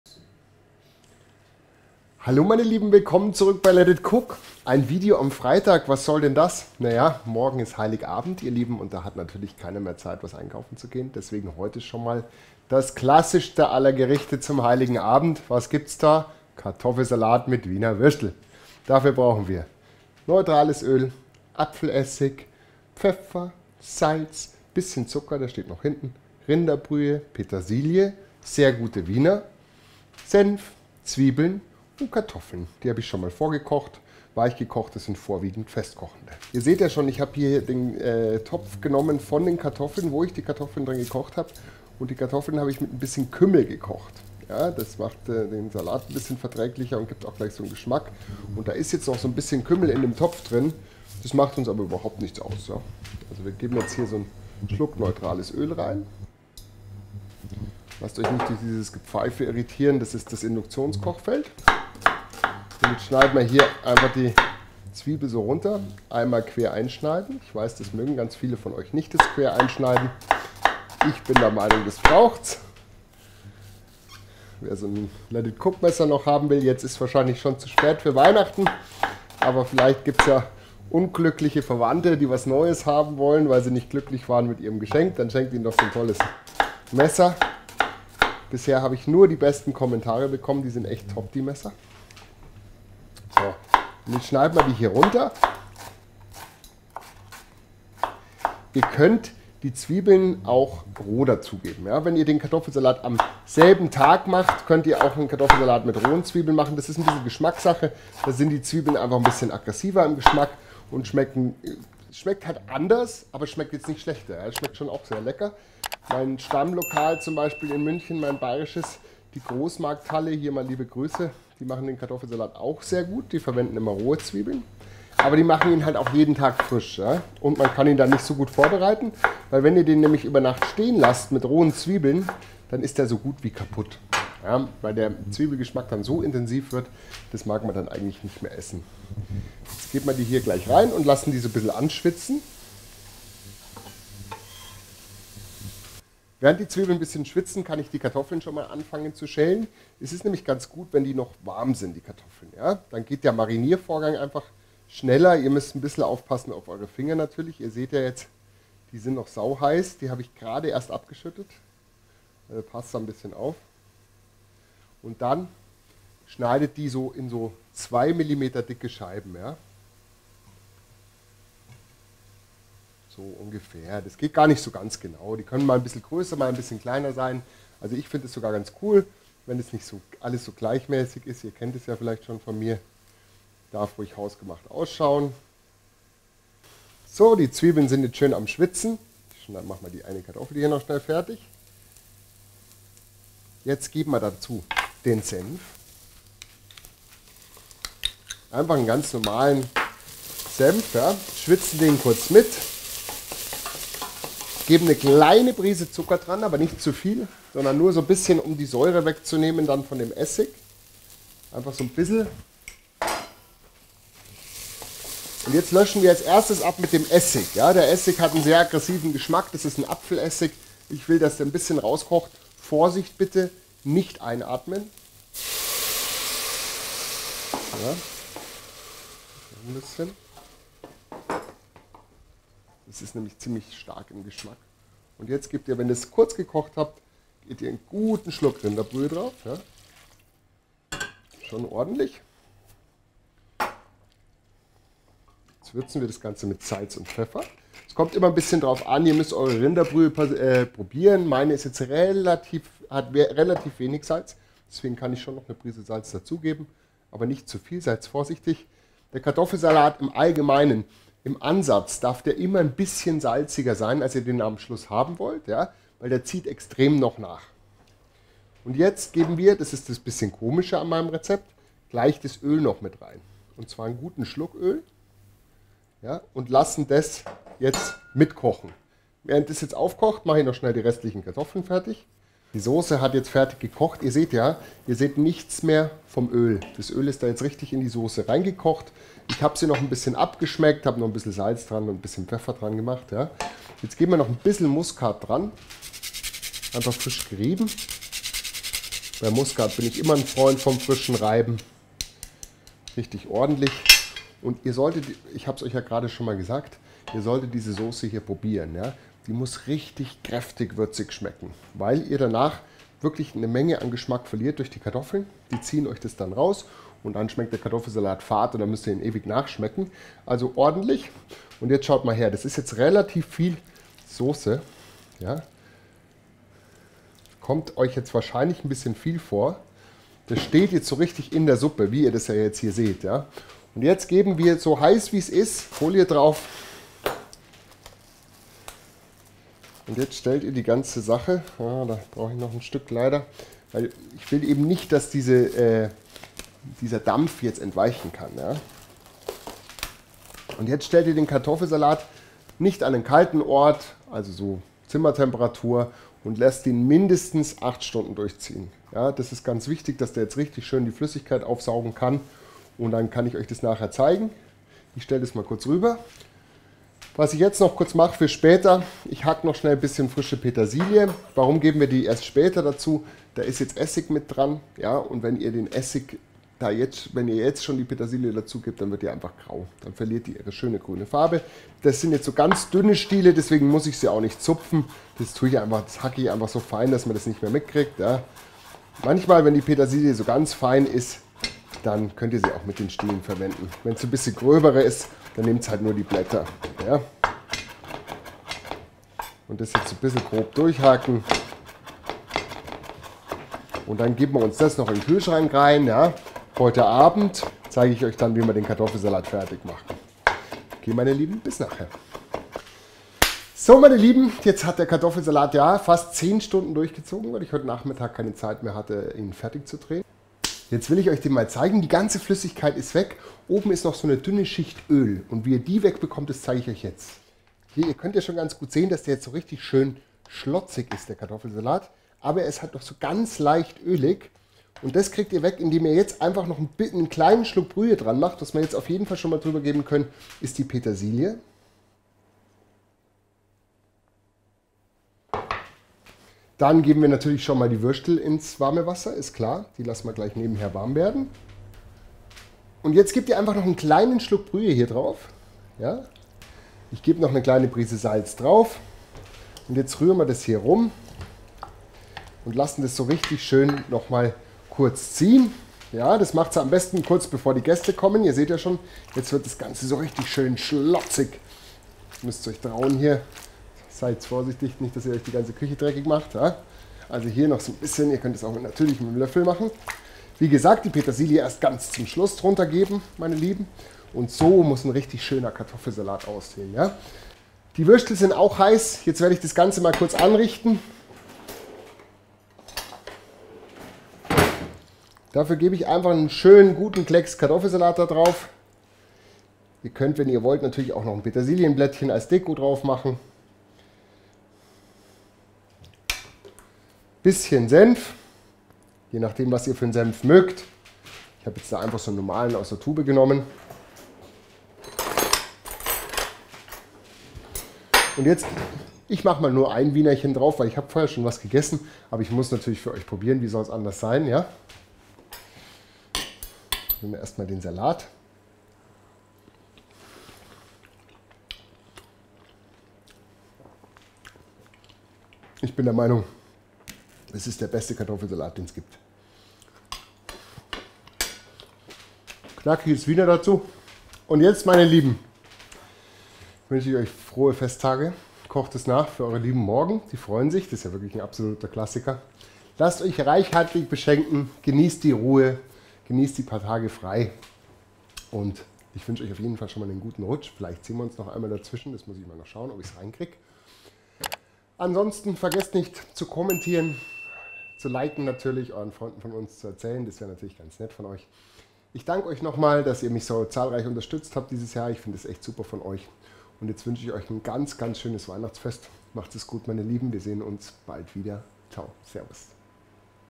Kartoffelsalat mit Wiener / Frohe Weihnachten
Hallo zusammen, hier sind die Mengenangaben für den absoluten Klassiker: Kartoffelsalat mit Wiener, gutes Gelingen und guten Appetit! Vielleicht gefällt euch auch dieses Rezept: Ravioli

Zutaten für 6-8 Personen:

2000 g Kartoffel (vorwiegend festkochend)
2 Stk Zwiebel

2 EL Petersilie (gehackt)
3 EL mittelscharfer Senf
400 ml Brühe (Rinder/Hühner / Gemüse)
150 ml neutrales Öl
120 ml Apfelessig
1 Prise Muskatnuss
1 Prise Zucker
  Salz (zum Abschmecken)
  Pfeffer (zum Abschmecken)

Sumo 19
Sennheiser Ton